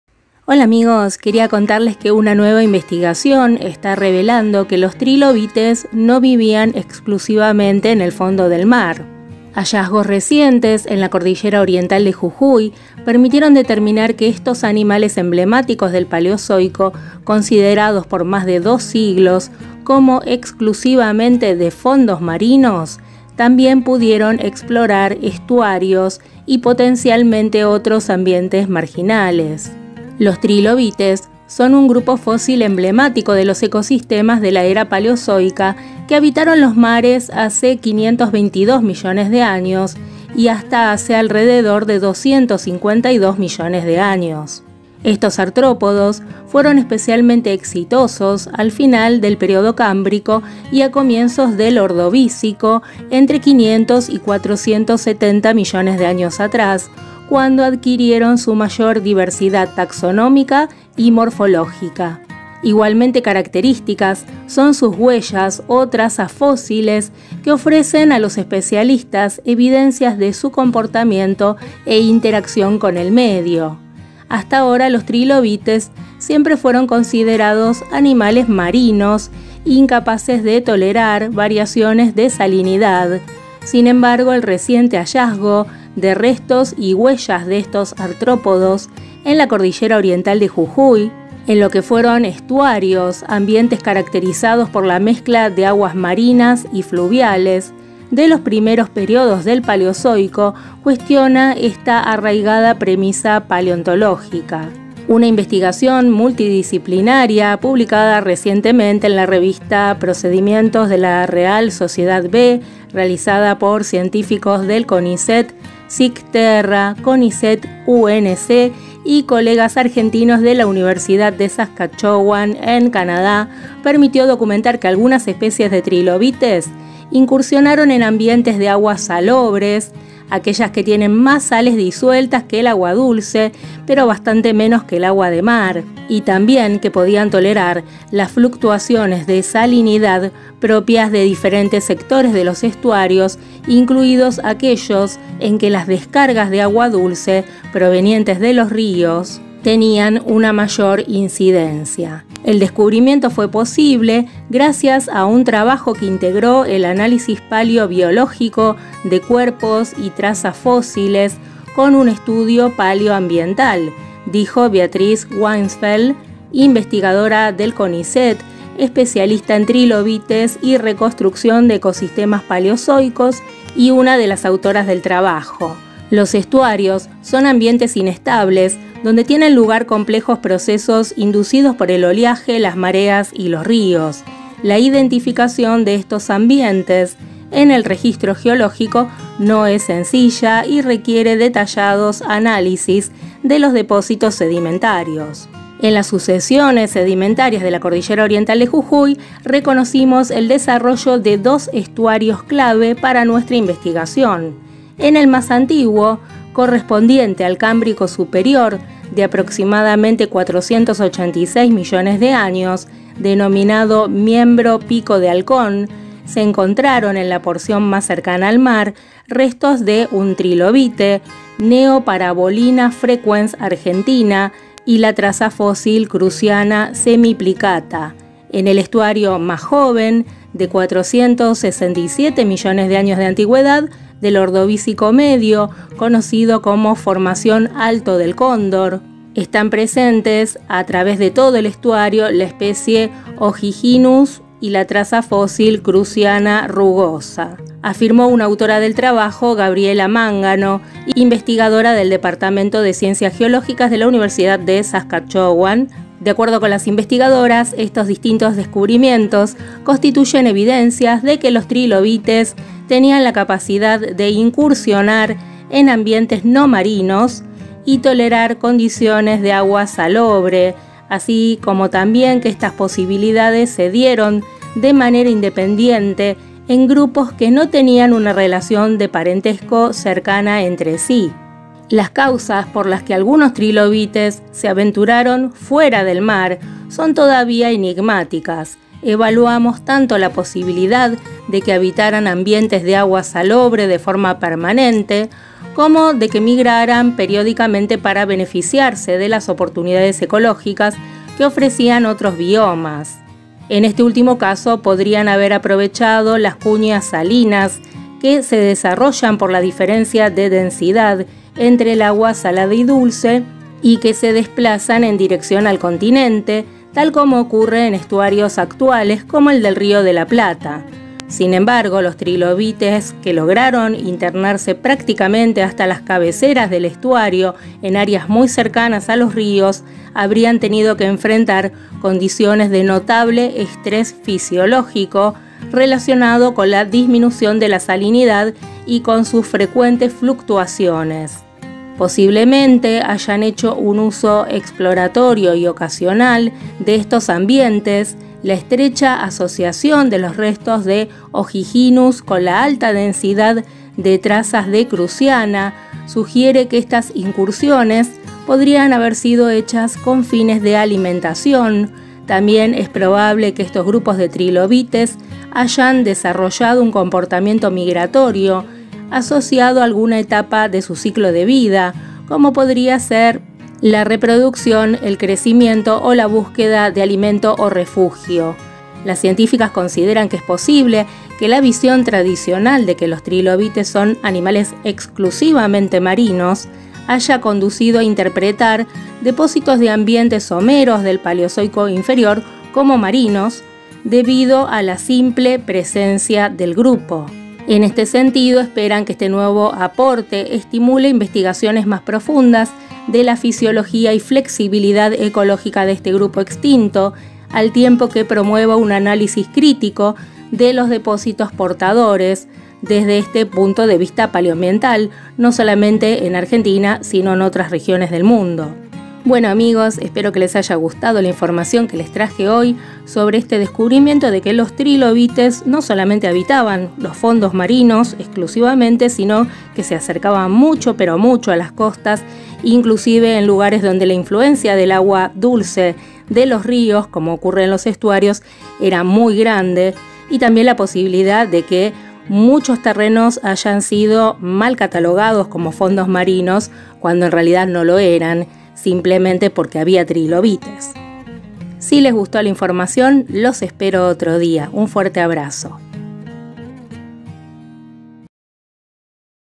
¿Nos equivocamos con los Trilobites? 😳😳😳
Tradicionalmente, los lugares donde aparecían trilobites se clasificaban como "antiguos fondos marinos" ... pero ahora eso se pone en debate...

Información: CONICET